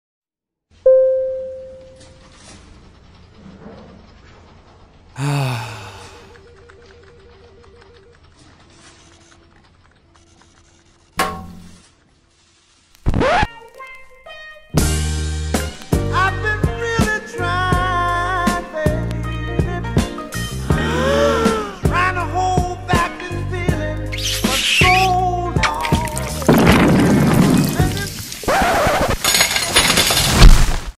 "Fizzed Up" Animated Short
Fizzed up a rom com gone wrong. When vending machines fall in love. First year final project. Team consisted of Matthew Duddy, Glenn O Neil, Lauren Bell and David Pollock